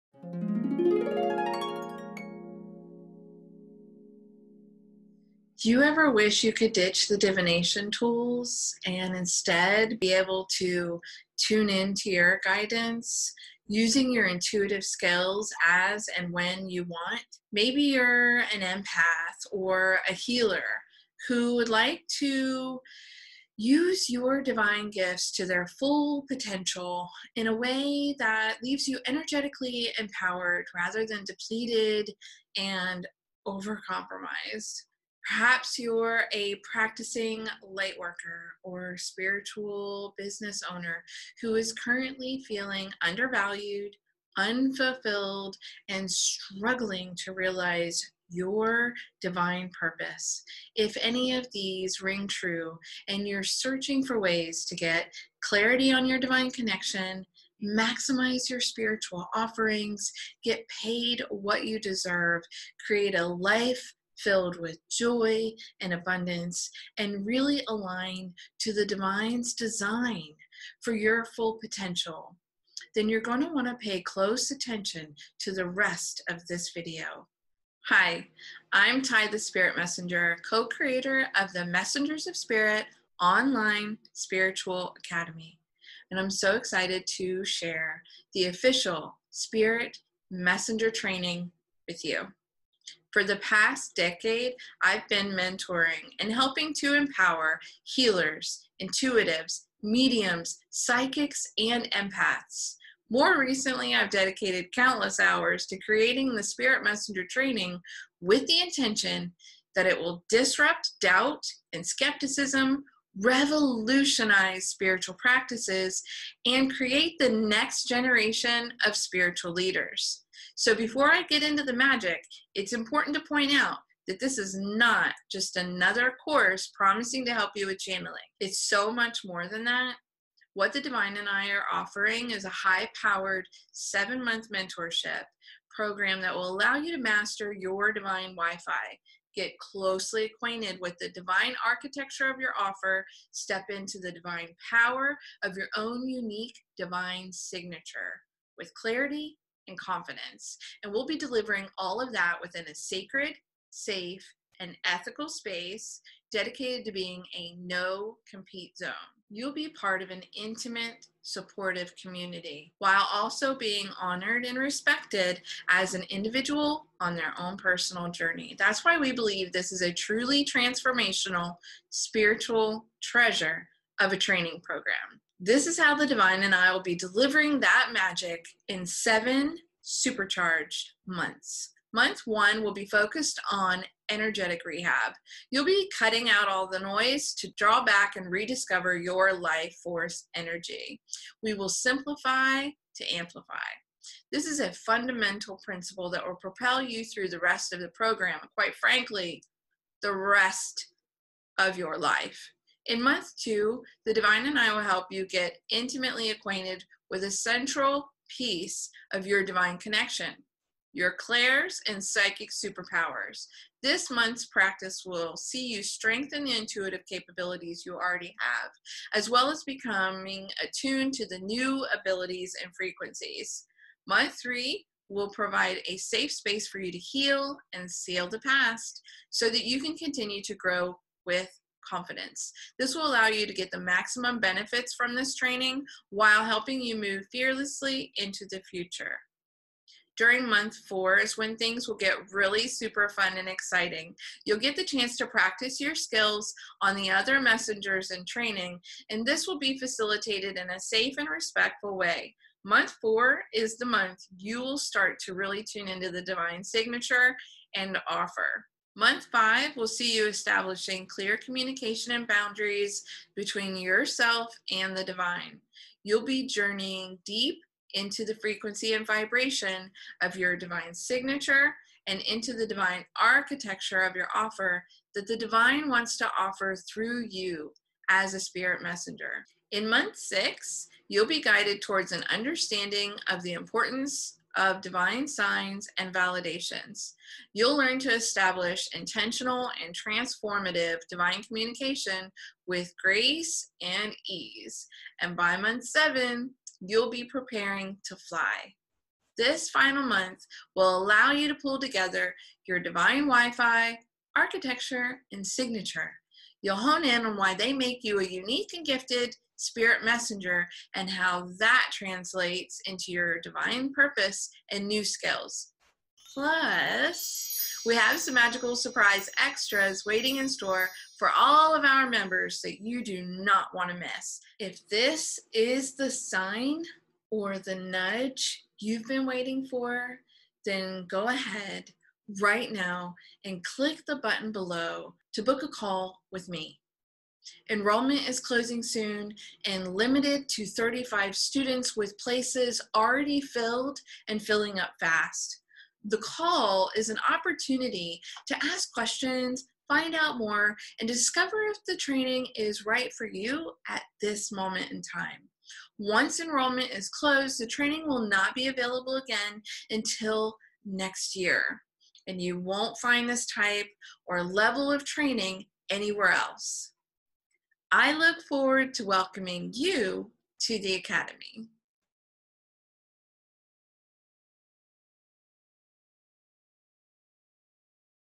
Activate your mission and discover your unique gifts!
In this video, Ty Gold | Alchemist talks about the mentorship opportunities for Empaths, Healers, Mediums and Intuitives, the Spirit Messenger Training and Mastery. The Spirit Messenger Training is the most reliable way to install your divine wifi. It promises to supercharge your connection with your Divine Support Team, so that you can serve at a greater capacity, save your resources, and shine your authentic light.